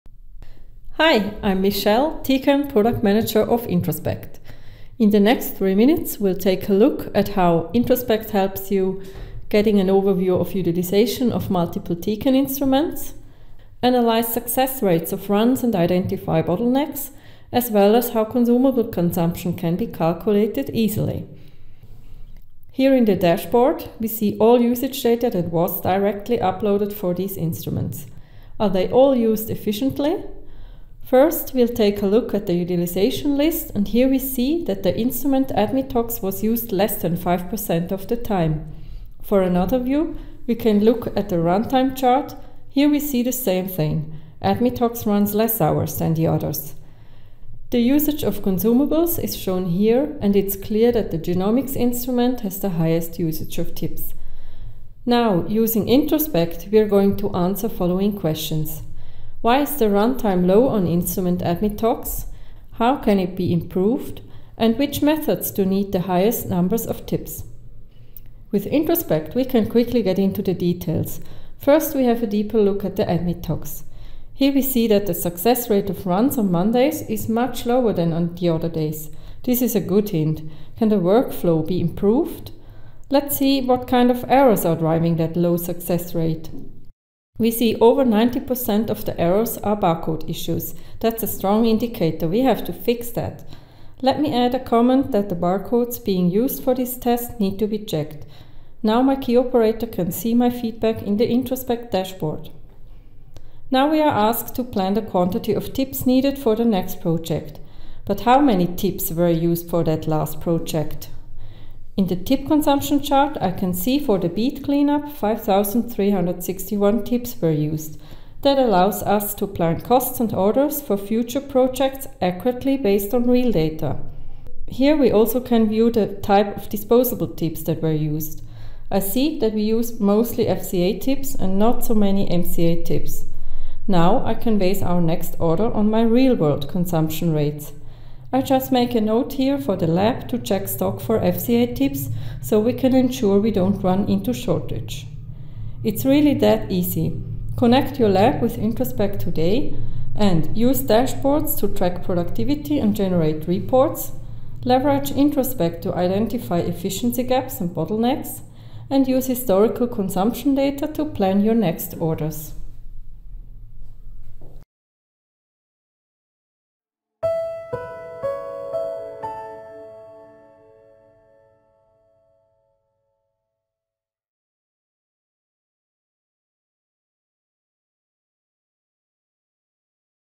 Tutorial shows how usage data will be tracked with Introspect
This free tutorial shows you how usage data of connected Tecan instruments will be tracked with Introspect. It shows how:
• You get an overview of utilization of multiple instruments
• Analyze success rates of runs and identify bottlenecks
• As well as how consumables consumption can be calculated easily
and more.